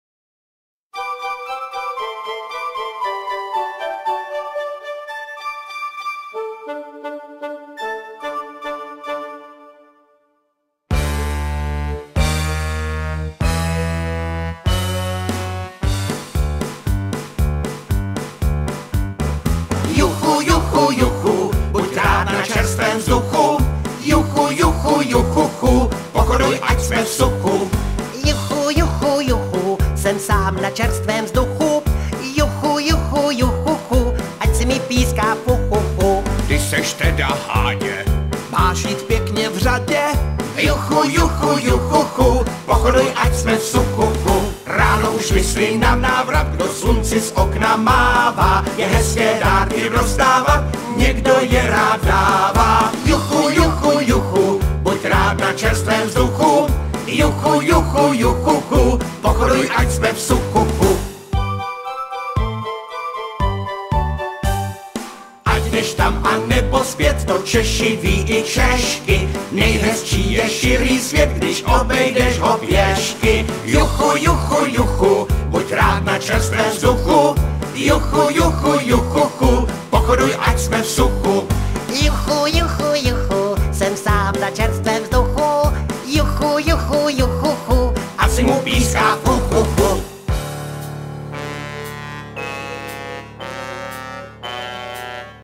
Sněhurka na ledě - přípravy
Nahlédněte do zákulisí příprav nově vznikajícího muzikálu Sněhurka na ledě